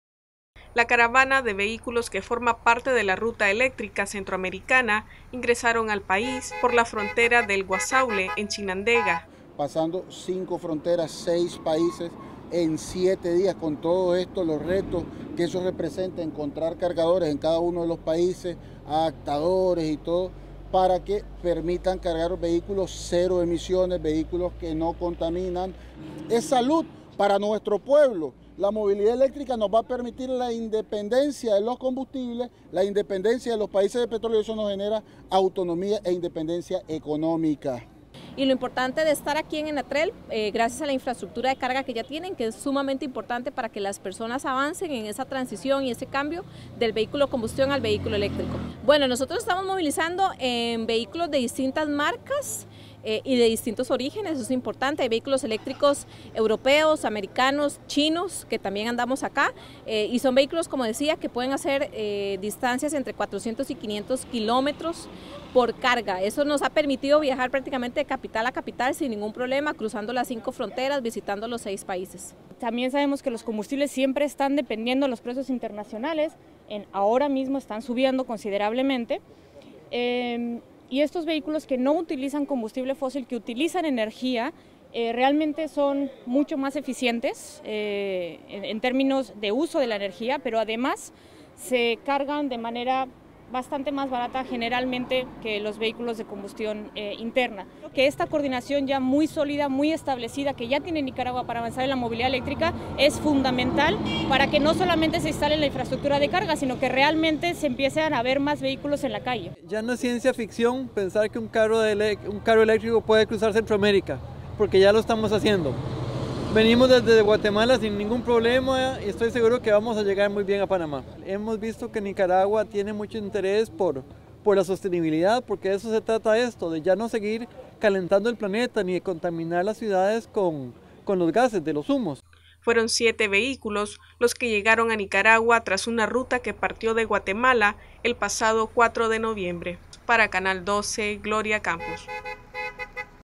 Noticias12Nicaragua Nicaragua recibe caravana centroamericana de vehículos eléctricos
Nicaragua recibe caravana centroamericana de vehículos eléctricos